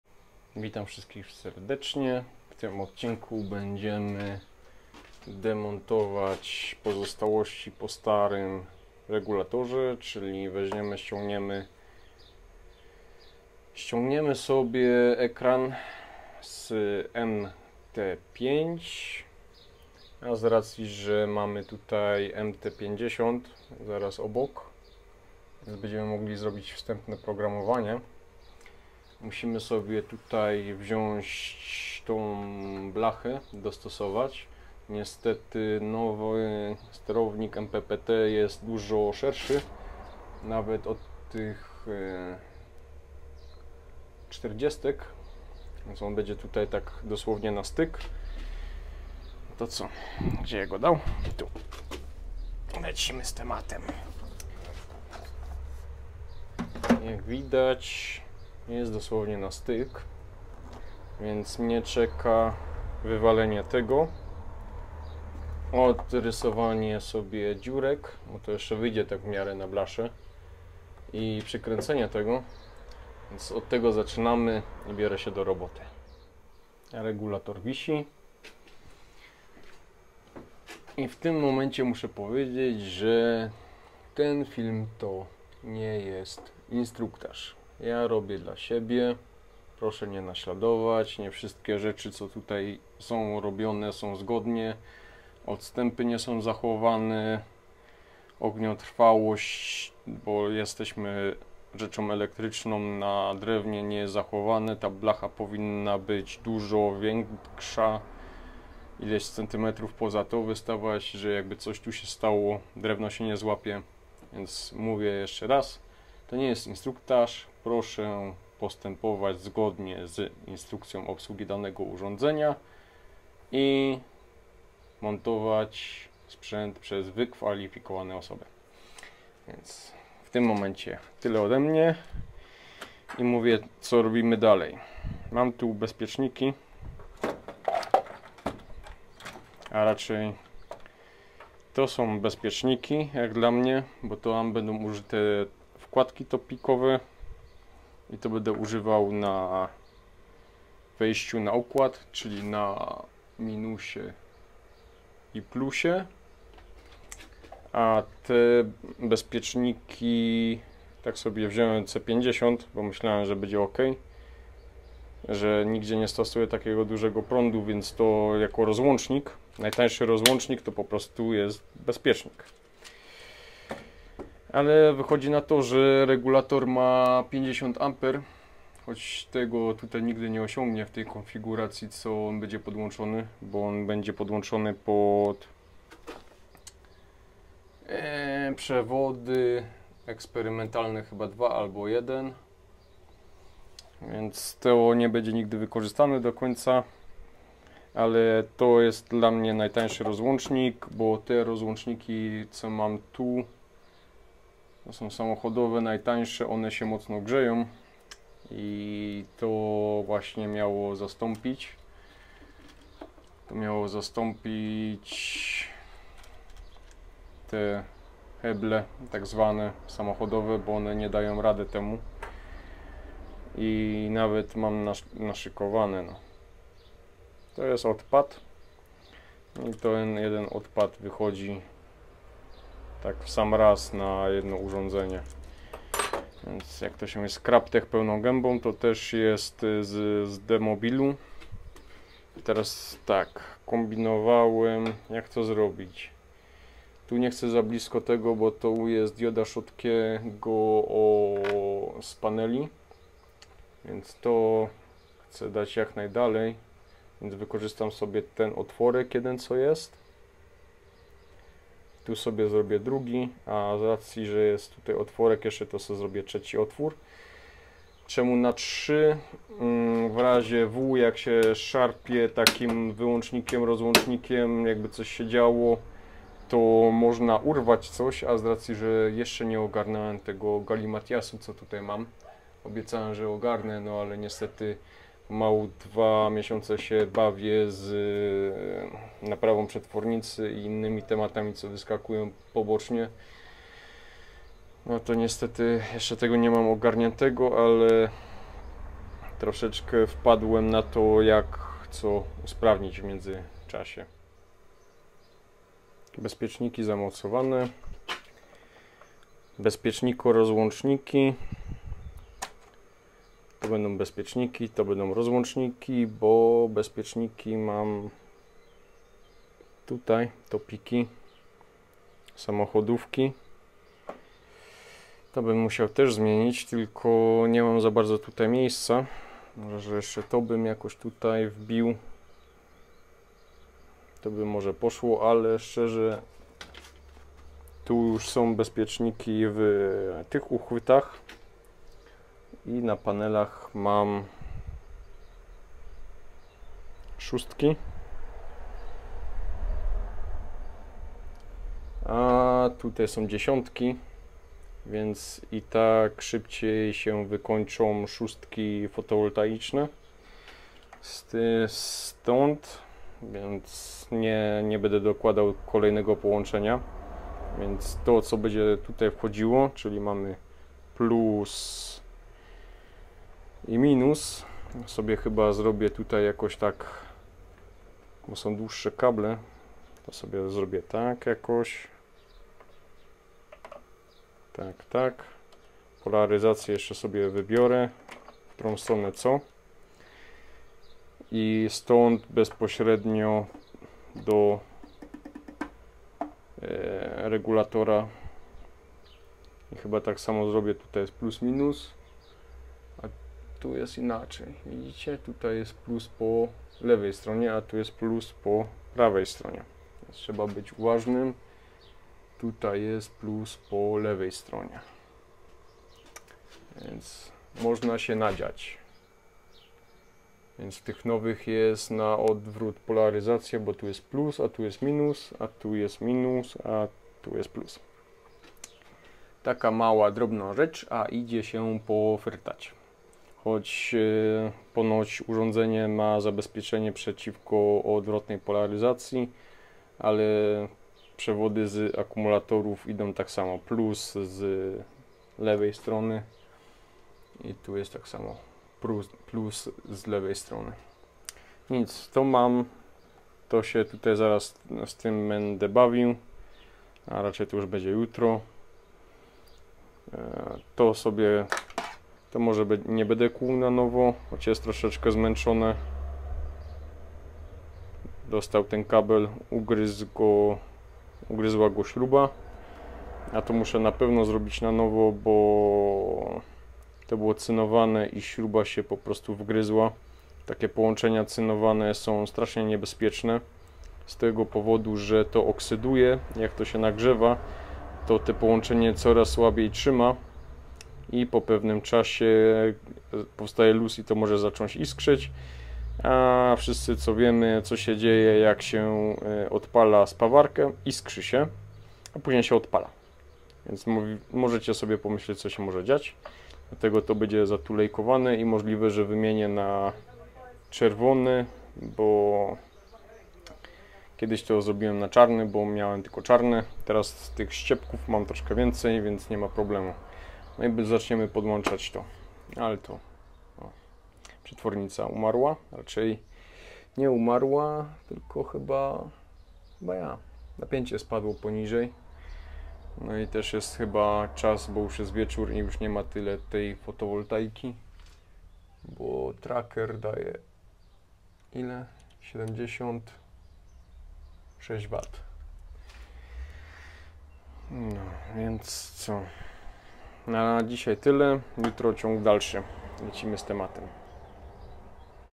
OFF GRID MPPT Tracer 5420AN - Montaż regulatora PV część 1
Linki do materiałów użytych w filmie:
Używając linków afiliacyjnych wspomagasz kanał, dziękuje.

W tym odcinku nowy regulator MPPT do mojego systemu off grid. Ostatnio rozpocząłem regeneracje jednego z najstarszych moich regulatorów co kupiłem, jednostka #2 była zakupiona używana więc nie wiem w jakich warunkach pracowała i jak ciężko miała ale na pewno był czas na „remont”.

Czas montażu regulatora nastał więc od porządków trzeba zacząć więc zbędne sprzęty musiałem zdemontować i przygotować miejsce do montażu nowego regulatora MPPT. Dałem sobie kilka dni by przemyśleć sytuacje jak co dać i jak podłączyć. Ważne jest to by konfiguracja jaką teraz zastosuje była na tyle elastyczna bym mógł w przyszłości ten sposób „klonować”
Plan to podstawa, tak by nie musiałem w przyszłości poprawiać to co zostanie zrobione.

Timeline:
0:00 - 1:16 Co będzie robione
1:17 - 2:09 Sprostowanie
2:10 - 5:05 Plan na bezpieczniki
5:06 - 9:16 Plan na okablowanie

Aktualny odcinek wykonany za pomocą:
Synco - G1L - mikrofon
IPhone 8 - Audio i wideo
Montaż - iPad 9